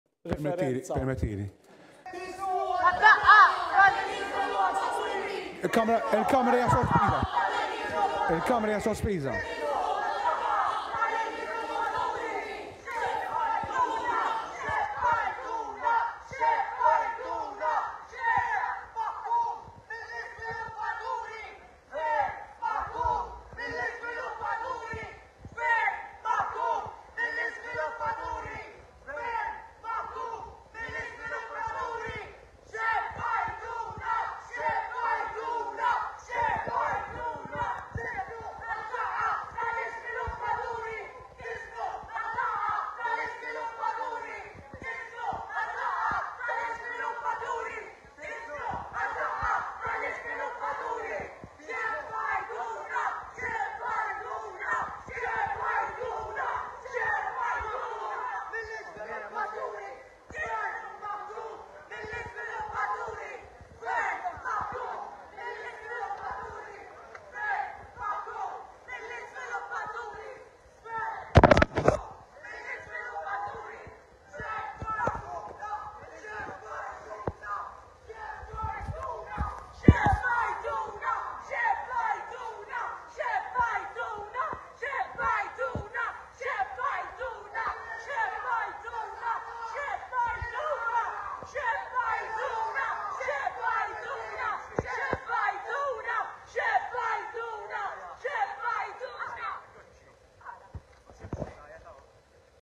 Activists disrupt Budget speech, protest against unbridled development
Activists from Graffitti interrupted Malta's Budget 2025 speech, protesting against rampant development. Wearing pig masks, they chanted criticisms and threw flyers, prompting a brief suspension of parliament. Graffitti condemned the influence of powerful developers over politics, alleging environmental harm and inadequate planning reforms, especially around Malta’s Villa Rosa project. The NGO criticized authorities for ignoring illegal construction rulings and failing to engage with public concerns.